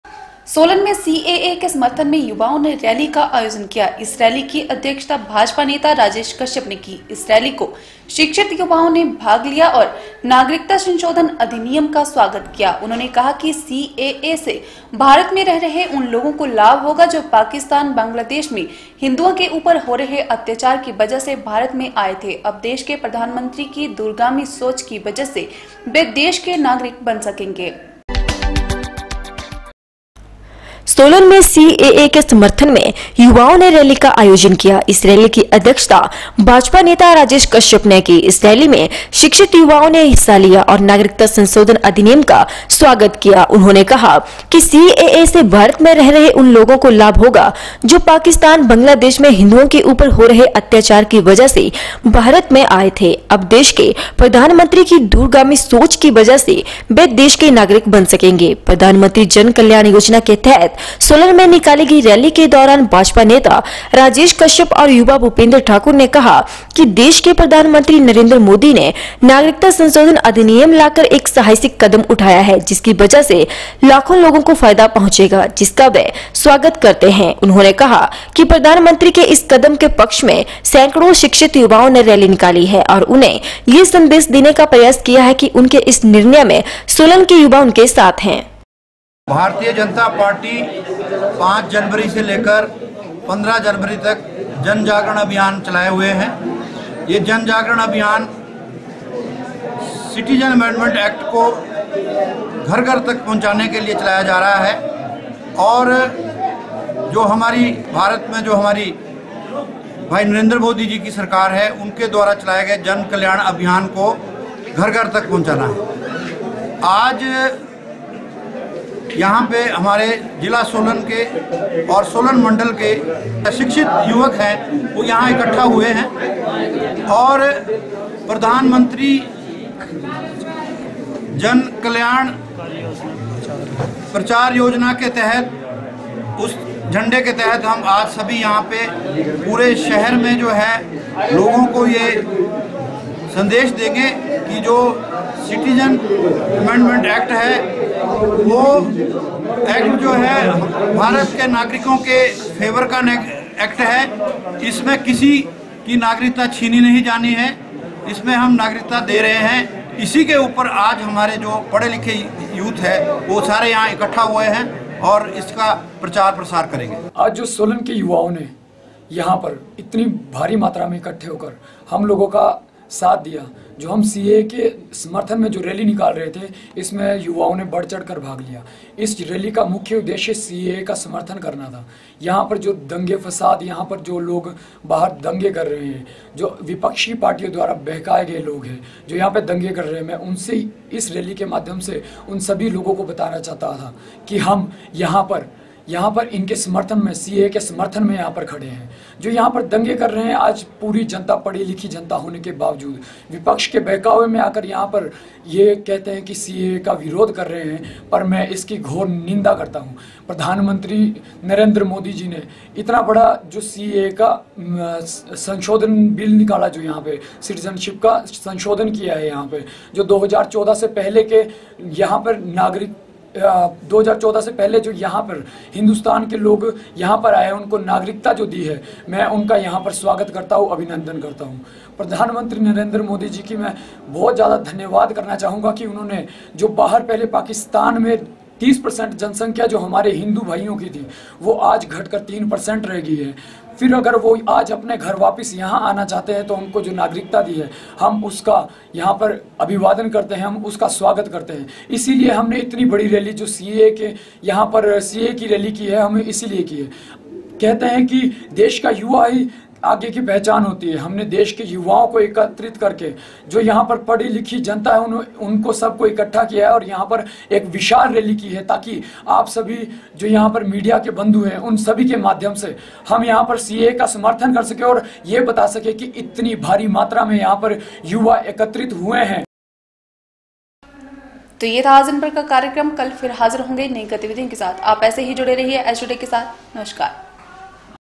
8 JAN N 16 END सोलन में सी ए ए के समर्थन में युवाओं ने रैली का आयोजन किया
सोलन में  सी  ए ए के समर्थन में युवाओं ने रैली का आयोजन किया | इस रैली की अध्यक्षता भाजपा नेता राजेश कश्यप ने की | इस रैली में शिक्षित युवाओं ने भाग लिया और नागरिकता संशोधन अधिनियम का स्वागत किया | उन्होंने कहा कि सी ए ए से भारत में रह रहे उन लोगों को लाभ होगा जो पाकिस्तान बांगलादेश में हिन्दुओं के उपर हो रहे अत्याचार की वजह से भारत में आए थे अब देश के प्रधान मंत्री की दूरगामी सोच की वजह से वह देश के नागरिक बन सकेंगे |